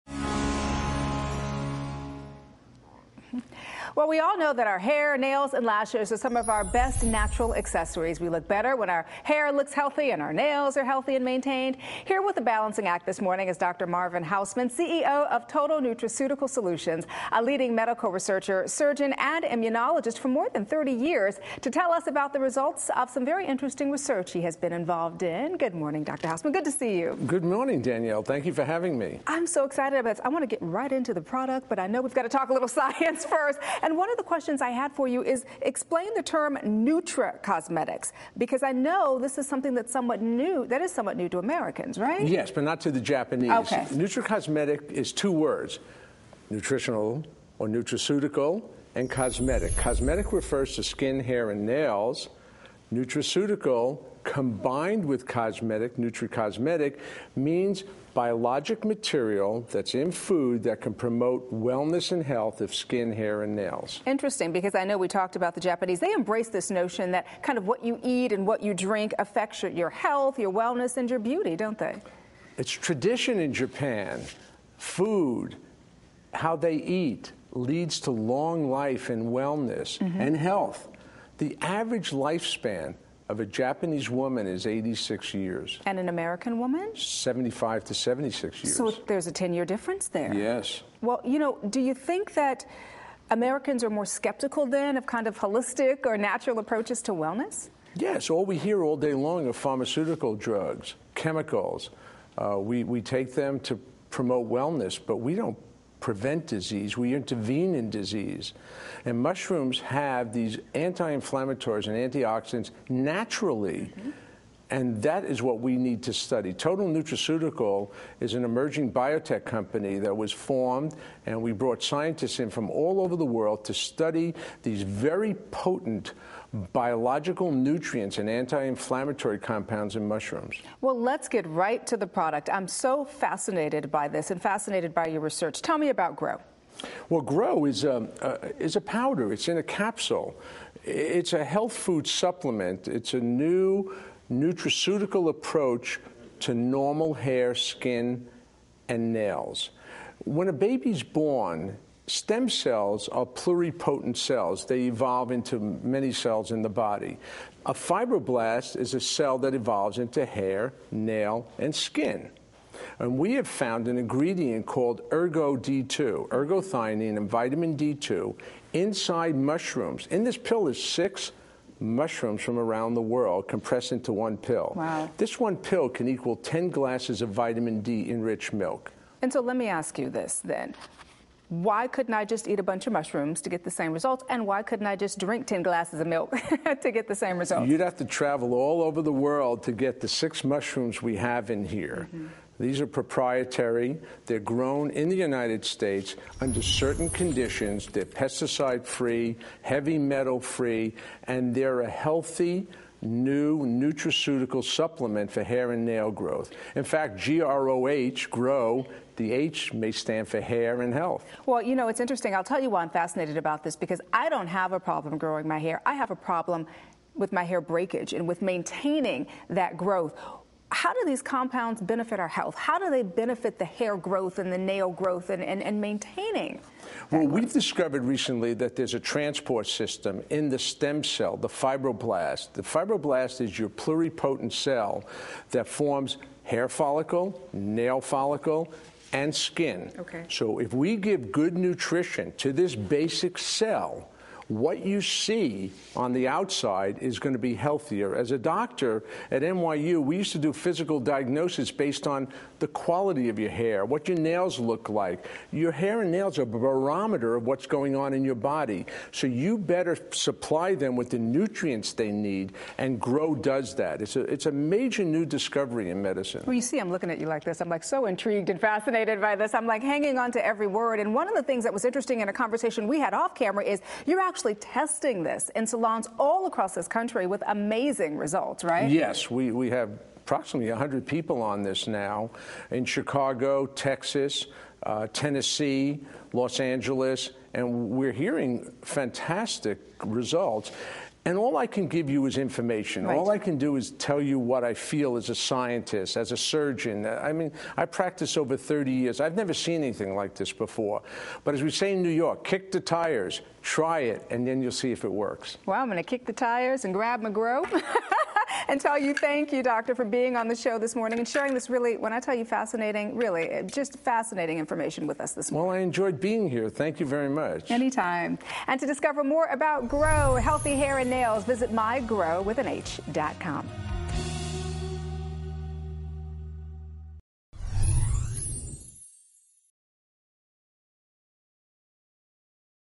Total Nutraceutical Solutions, Inc.
Join Dr. Marvin Hausman to discuss a surprising discovery that resulted from a research project he was conducting on health and mushrooms. Not only do select mushrooms contain anti-oxidants, they were found to contribute to healthier hair and nail growth. Becoming more widely known in the US are nutricosmetics; natural health products that work on the "Beauty from Within"